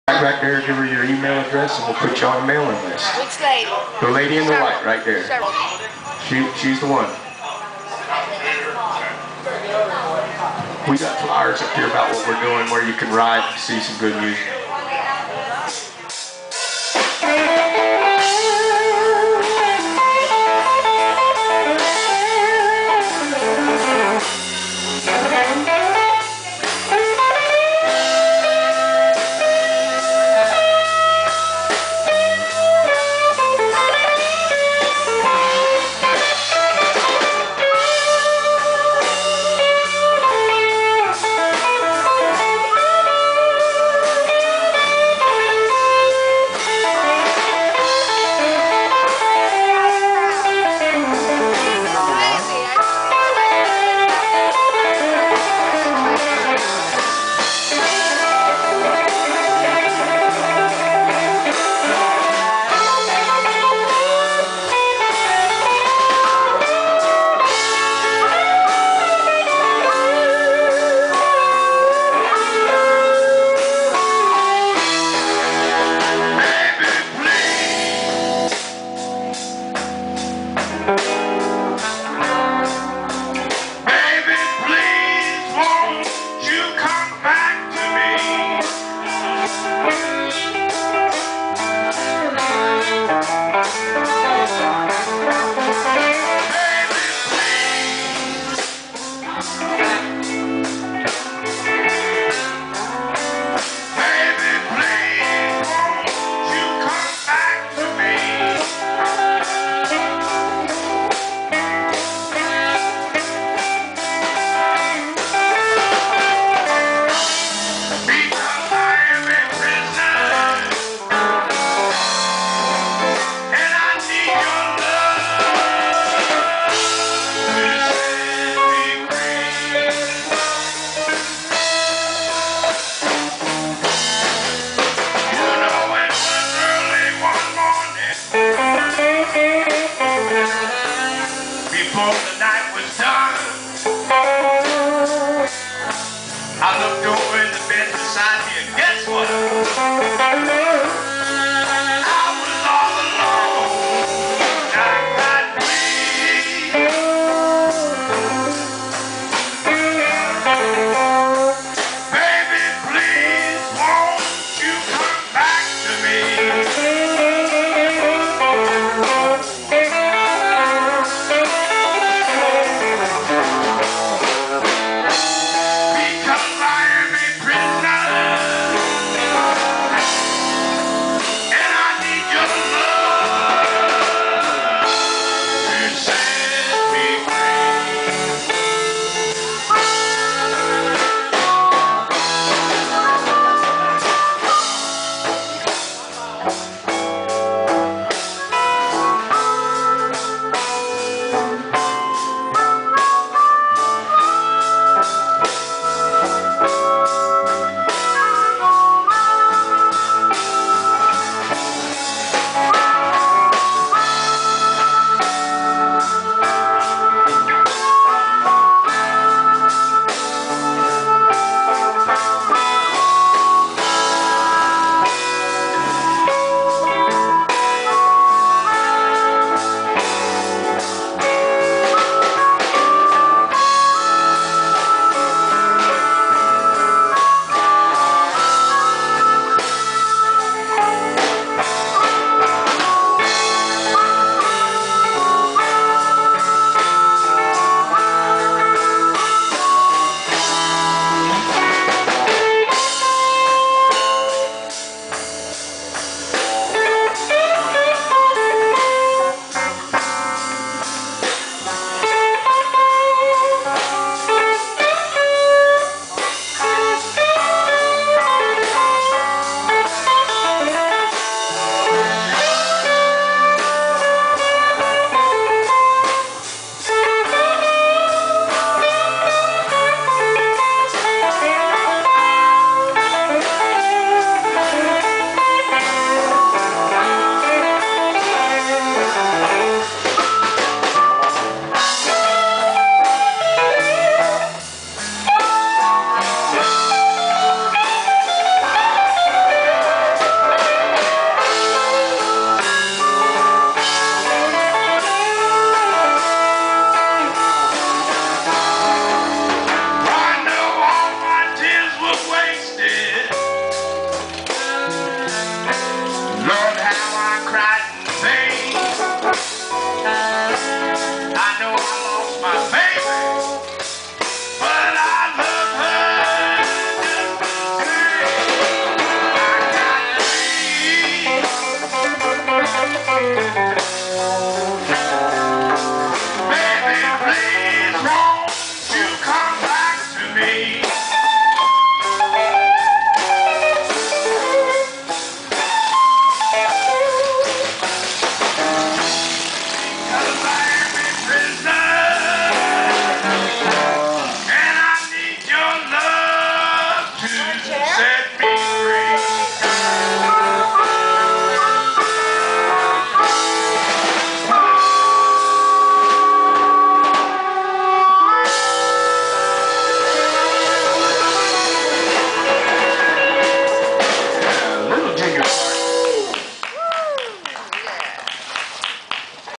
Howlin' Buzz
Local Blues band to Citrus County Fl. Recorded at Crystal River HD July 5,08